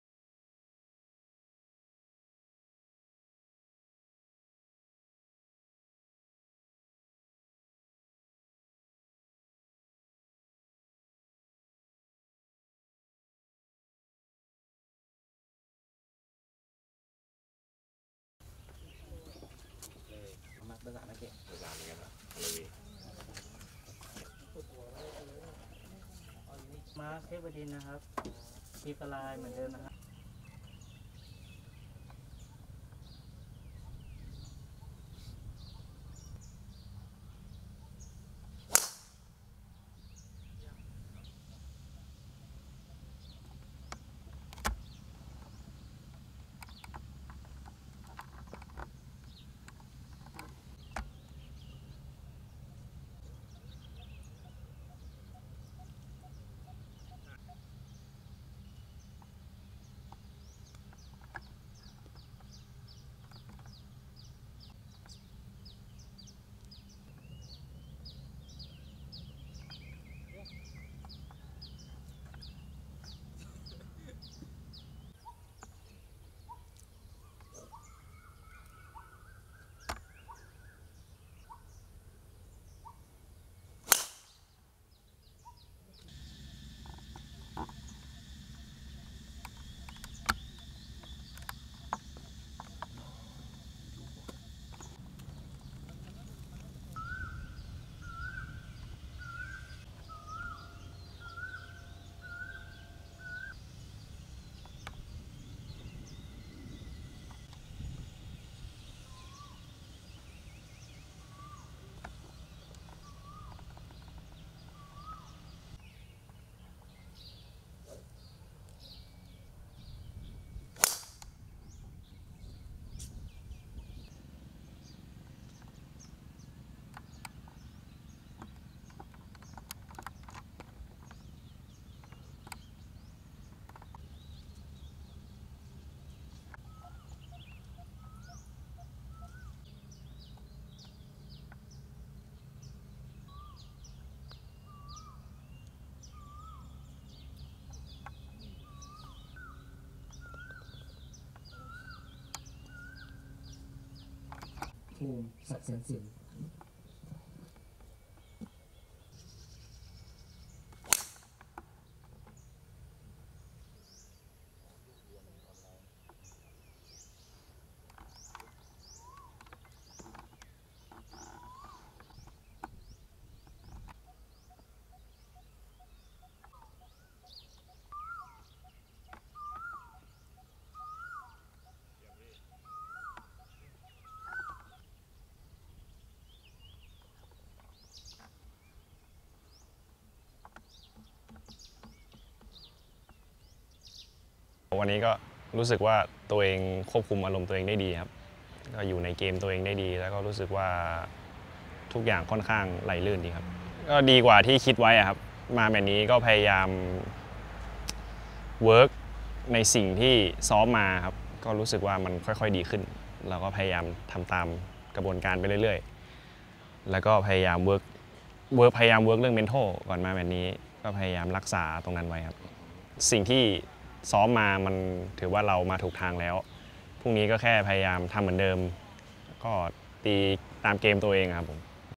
ภาพข่าวการแข่งขันกอล์ฟอาชีพสิงห์-เอสเอที ปราจีนบุรี แชมเปี้ยนชิพ 2022 วันทื่ 26 มีนาคม 2565
โคสุเก ฮามาโมโต
ผู้นำการแข่งขันกอล์ฟอาชีพสิงห์-เอสเอที ปราจีนบุรี แชมเปี้ยนชิพ 2022